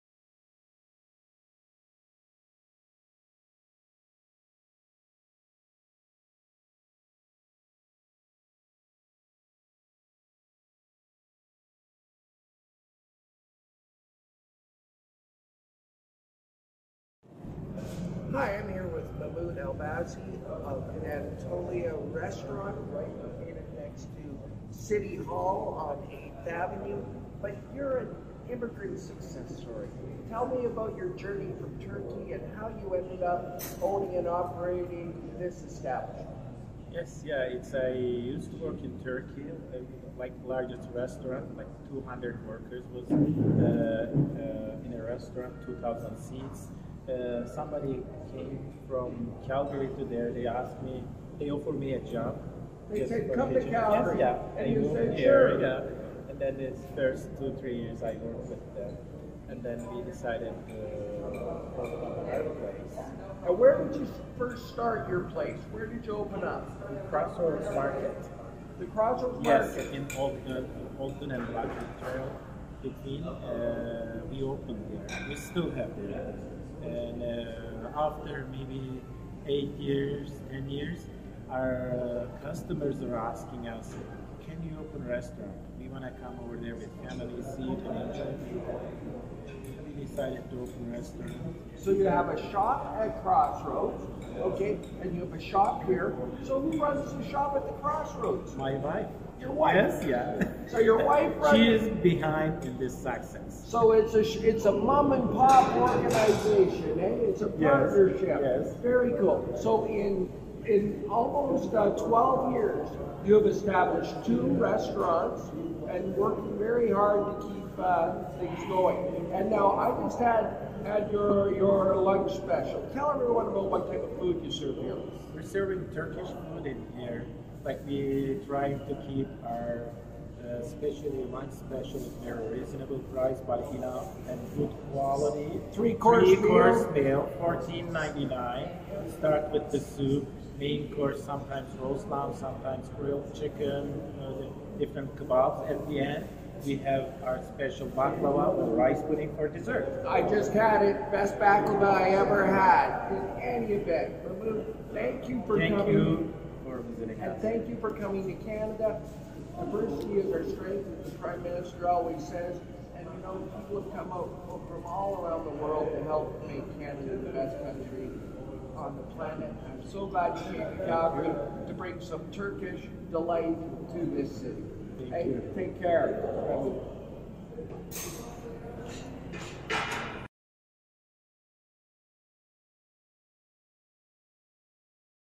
Anatolia Turkish Restaurant | SmallBusinessSunday | Kent Hehr
Anatolia Restaurant makes a good lunch special: bringing Turkish delight to the neighbourhood and enriching our city. I sat down with Mahmut, who came to Canada to be in the restaurant industry, and he and his wife are managing their business across two locations.

This SmallBusinessSunday, check out Anatolia Turkish Food and Mahmut.